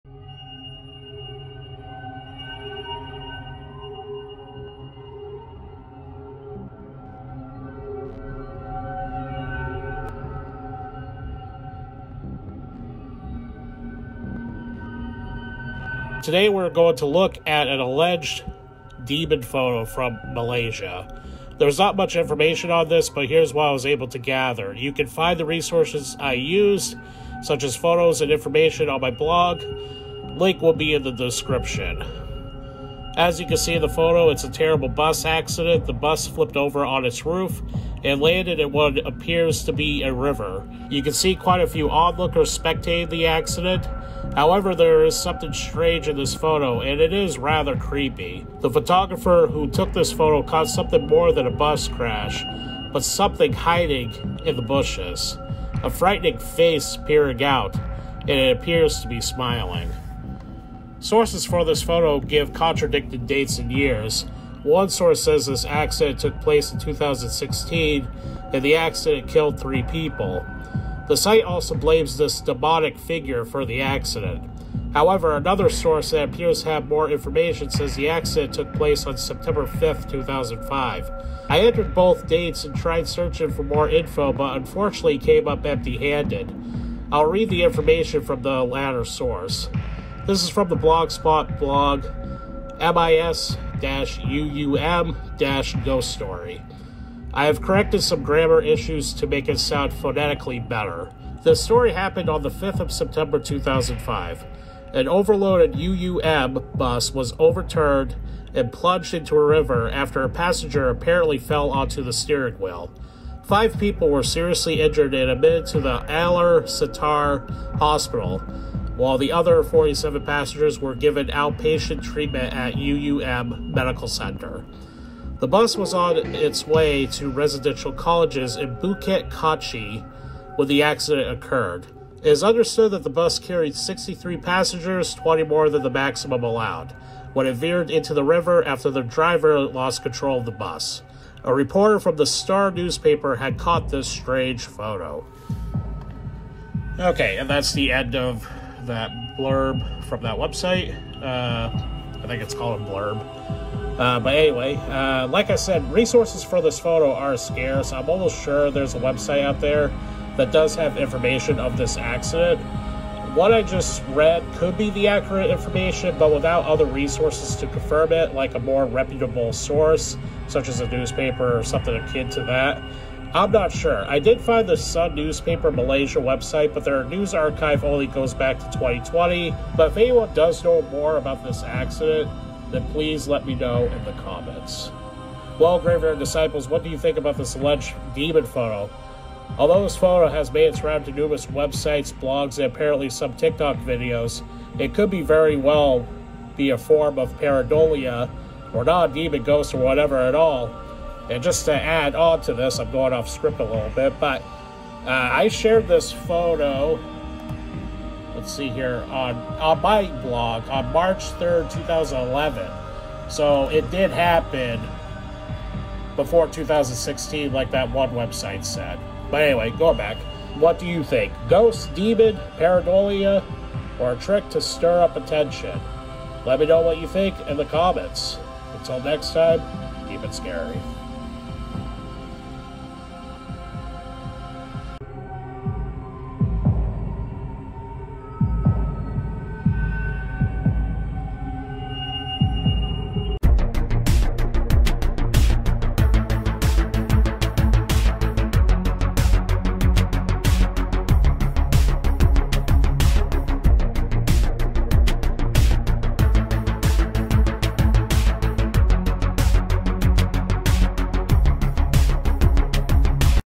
Paranormal Photo: Malaysian Bus Crash Demon
This photo has made its rounds over the years and some believe it is the photo of a demon that caused this horrific accident. Sources are scarce, but I tried my best to find some info on it. But what do you think? Is it a photo of a demon, pareidolia, or just a dude hanging out in the bushes?

Link to the article version with resources used for this video